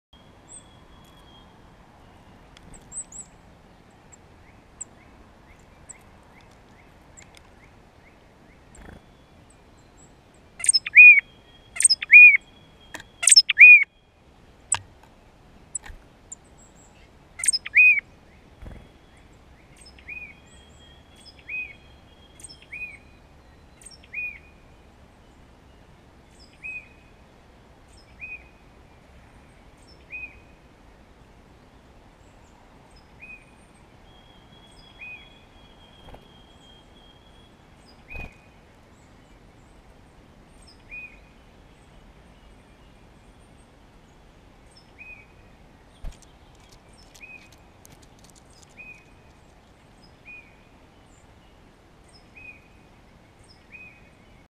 Tufted Titmouse call
Here is one of the tufted titmouses calls.